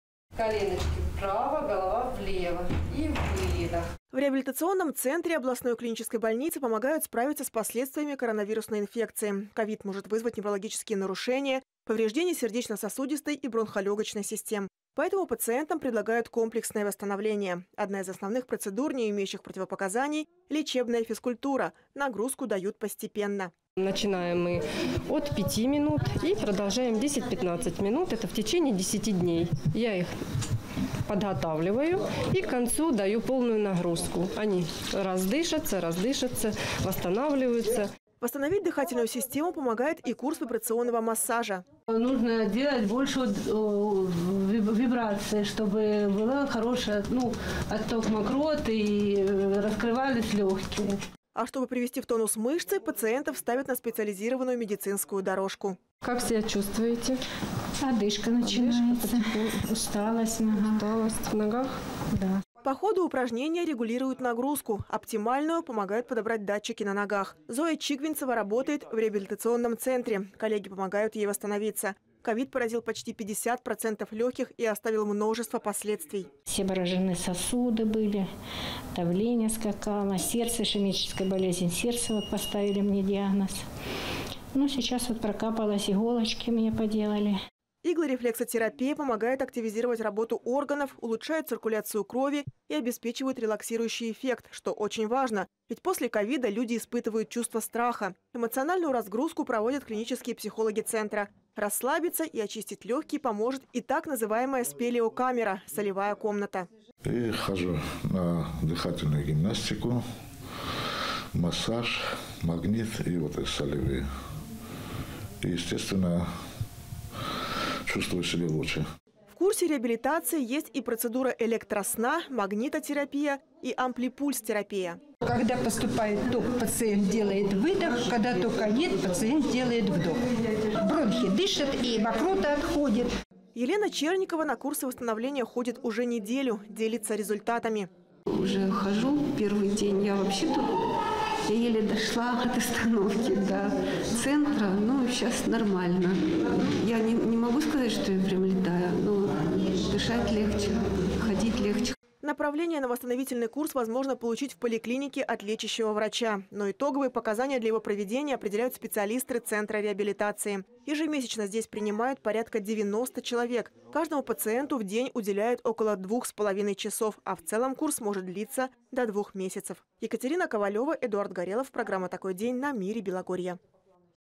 Реабилитация после COVID-19: что и как делать
С последствиями коронавируса помогают справиться в реабилитационном центре областной клиники.  Ковид может вызвать неврологические нарушения, повреждение сердечно-сосудистой и бронхолёгочной систем. Поэтому пациентам предлагают комплексное восстановление.
 Лечебная физкультура — одна из основных процедур, не имеющих противопоказаний.